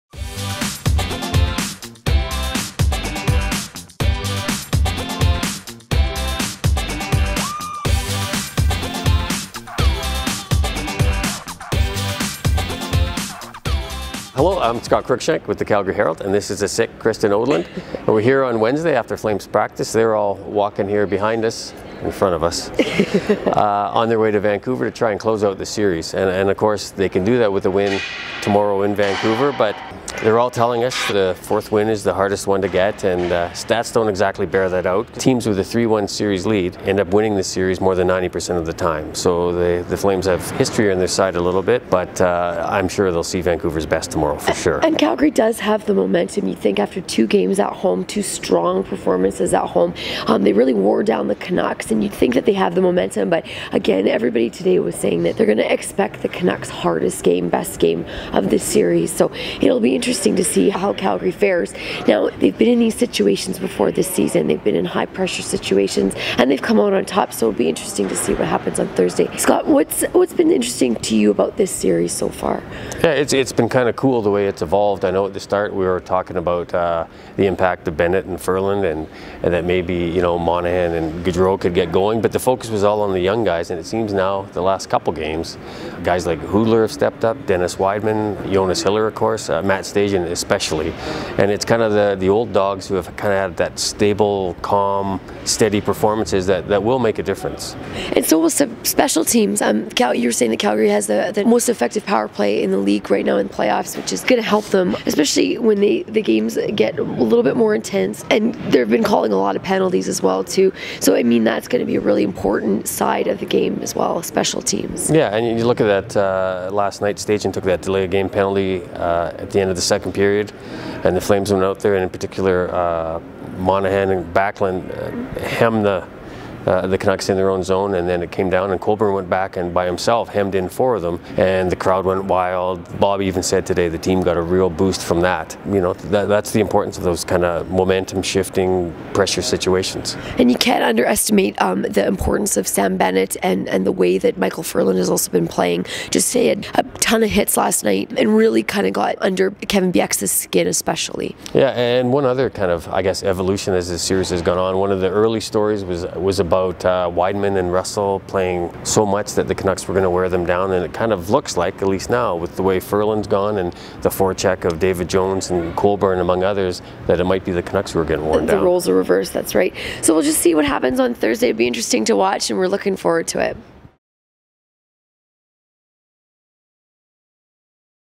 Benchwarmers April 22, 2015
Calgary Herald sports reporters Scott Cruickshank and Kristen Odland hit the bench for the latest Benchwarmers to discuss Tuesday nights Calgary Flames win over the Vancouver Canucks.